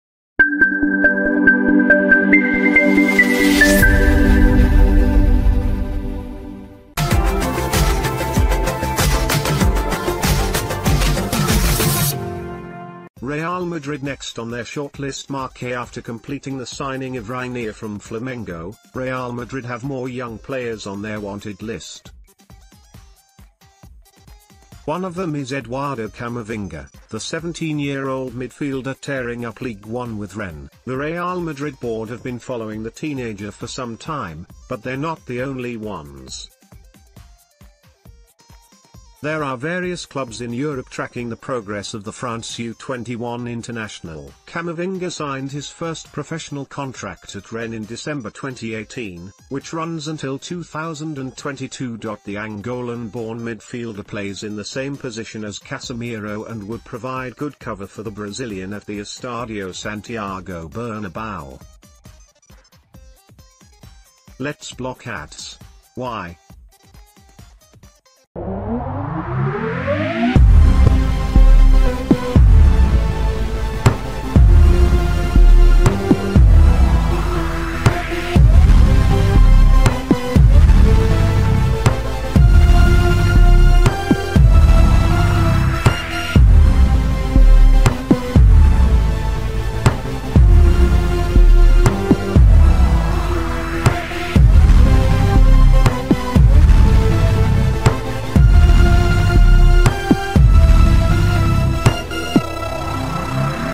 Real Madrid turn their attention to Camavinga- Real Madrid news today
Real Madrid Next on their shortlistMARCAAfter completing the signing of Reinier from Flamengo, Real Madrid have more young players on their wanted list. One of them is Eduardo Camavinga, the 17-year-old midfielder tearing up Ligue 1 with Rennes. The Real Madrid board have been following the teenager for some time, but they're not the only ones. There are various clubs in Europe tracking the progress of the France U21 international. Camavinga signed his first professional contract at Rennes in December 2018, which runs until 2022.The Angolan-born midfielder plays in the same position as Casemiro and would provide good cover for the Brazilian at the Estadio Santiago Bernabeu. Let's block ads! (Why?)